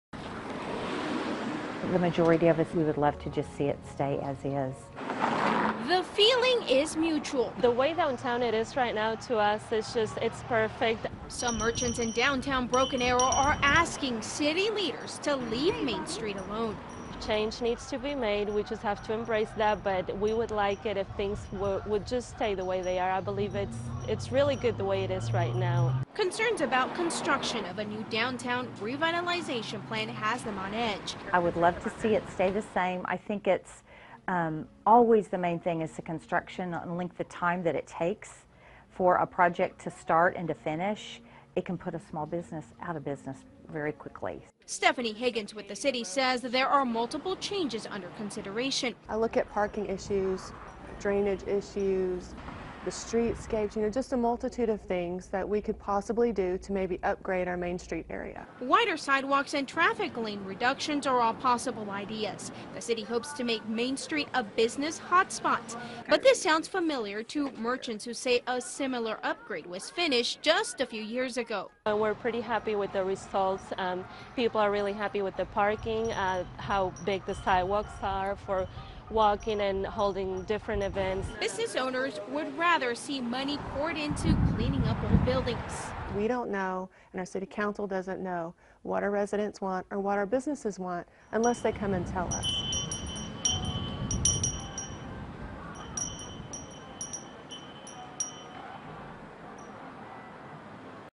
BA unveils revitalization ideas
Broken Arrow unveils ideas for downtown revitalization plans